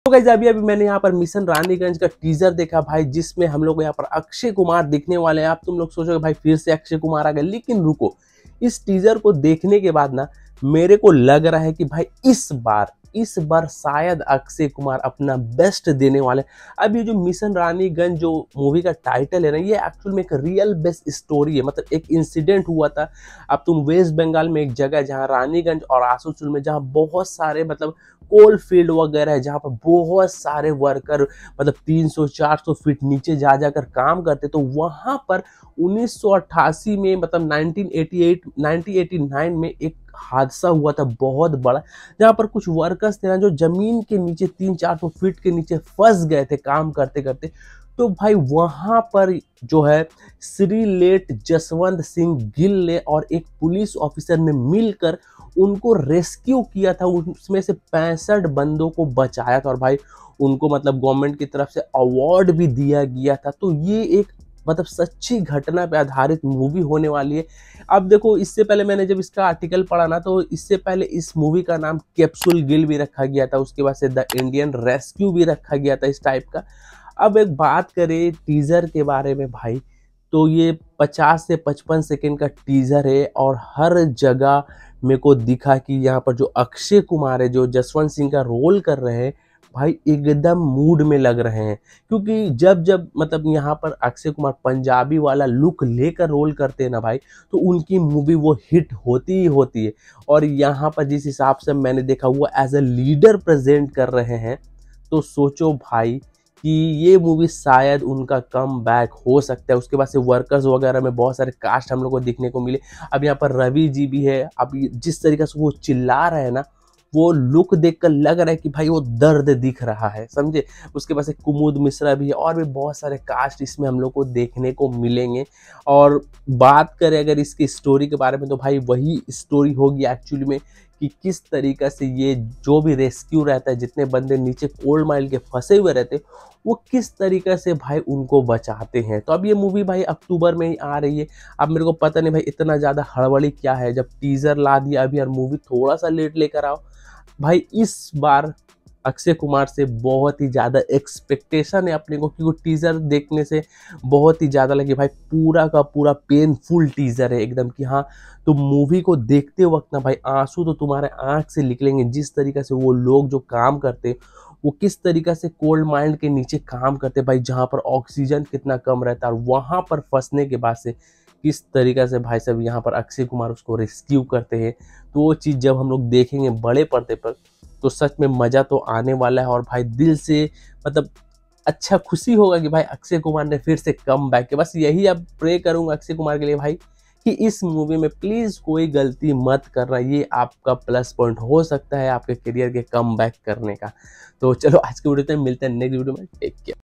Mission Raniganj Teaser Review | Akshay Kumar New Upcoming Movie Teaser Review | Baap Of Movies
Topics:
Mission Raniganj,
Mission Ranijanj teaser review,
Akshay Kumar Raniganj teaser,
Jaswant Singh Gill raniganj teaser review,